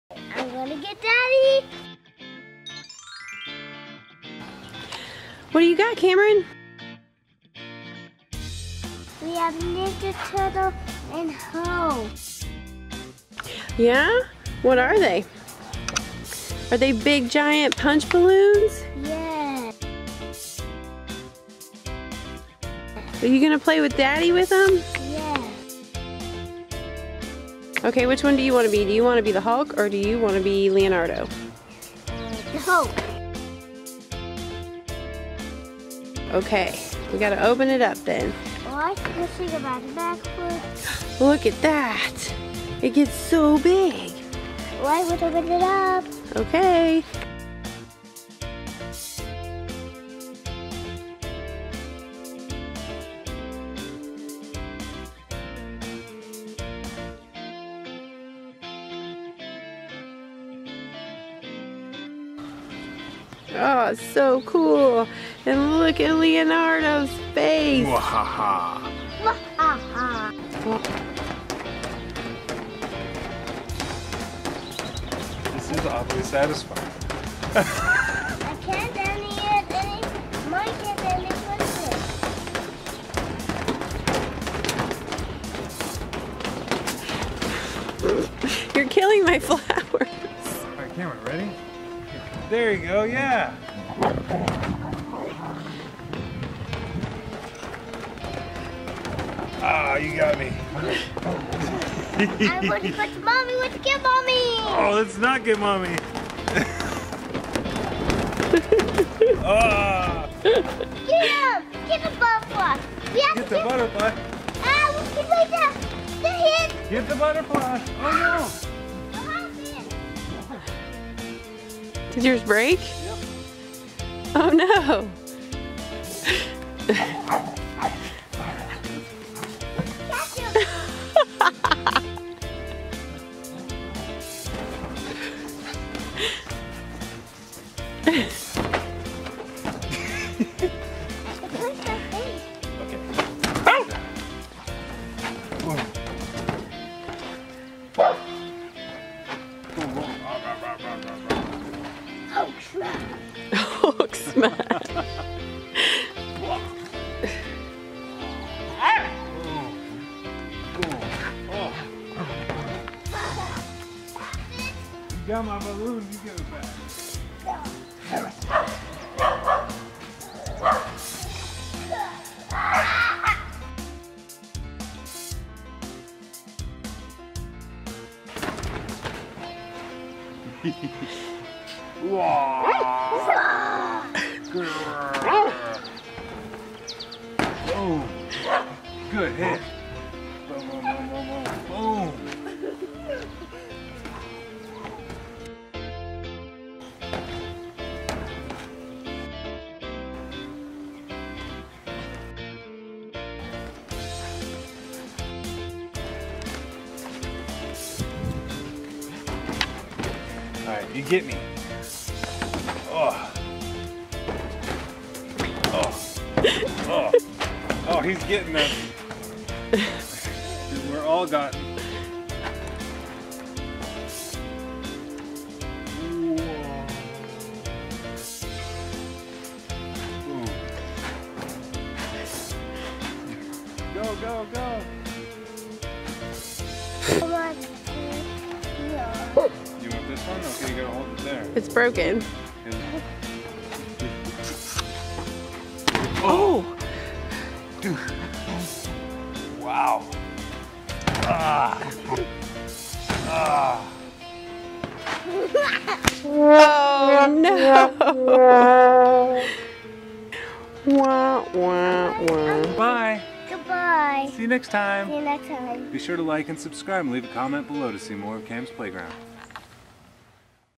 Punch Balloon
Punch Balloon. Daddy and Cam have a Hulk punch balloon and a ninja turtle punch balloon. They are going to blow them up and test them out. Come watch all the fun with these big balloons